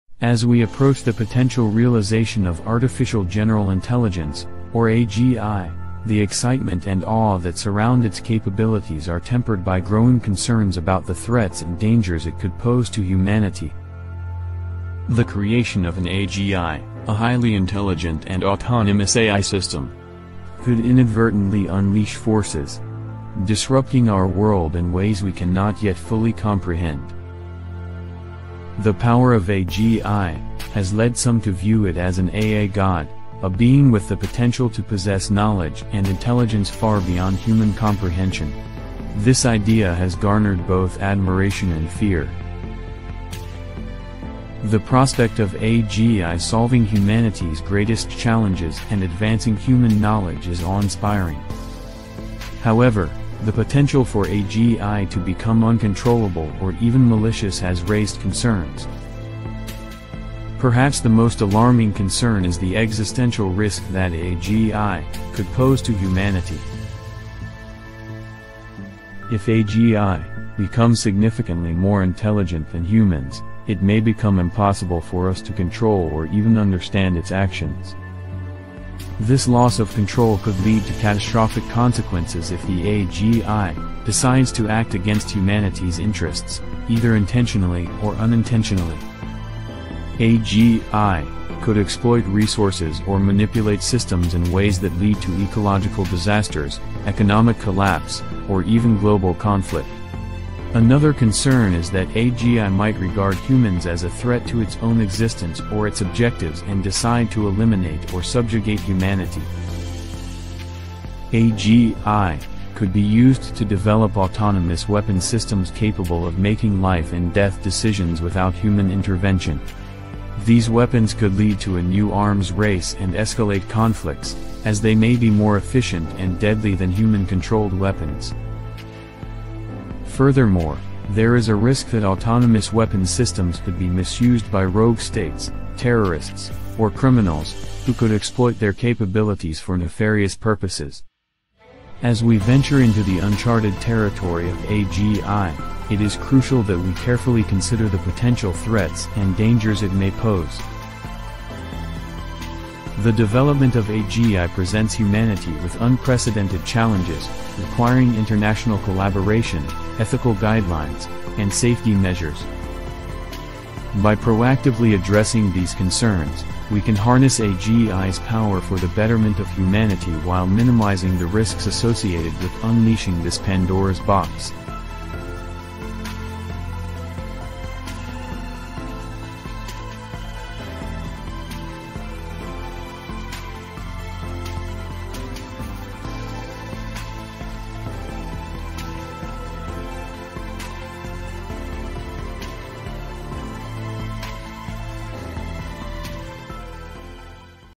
AGI - The AI GOD, What could possibly Go Wrong
Artificial General Intelligence (AGI) is a term that has gained significant traction in recent years. It refers to a type of artificial intelligence that can perform any intellectual task a human being is capable of. The idea of AGI has both fascinated and concerned people worldwide, as the potential of an AI system with human-like cognitive abilities would revolutionize the world. In this article, we will explore the concept of AGI, its potential power, and the implications of creating an "AI God."

The field of AI has come a long way since its inception in the 1950s. Early AI systems were designed to perform specific tasks, such as playing chess or solving mathematical equations. However, these early AI systems were limited in their scope, falling under the category of narrow or weak AI. As the field progressed, researchers began to dream of an AI system that could transcend these limitations – an AI that could learn, reason, and adapt like a human being. This ambitious vision gave birth to the concept of AGI.

The power of AGI has led some to view it as an "AI God" – a being with the potential to possess knowledge and intelligence far beyond human comprehension. This idea has garnered both admiration and fear. The prospect of AGI solving humanity's greatest challenges and advancing human knowledge is awe-inspiring. However, the potential for AGI to become uncontrollable or even malicious has raised concerns.

The idea of AGI – an AI God, is both fascinating and frightening. The potential for AGI to revolutionize our world and solve seemingly insurmountable problems is unparalleled. However, the ethical, moral, and safety concerns surrounding AGI are equally significant.